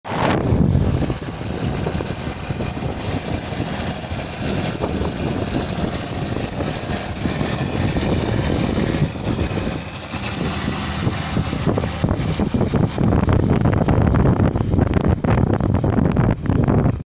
Orka po rozwożeniu obornika - John deere 6920S + Kuhn multi master + wał cambella expom krośniewice
Orka i rozwożenie obornika w tle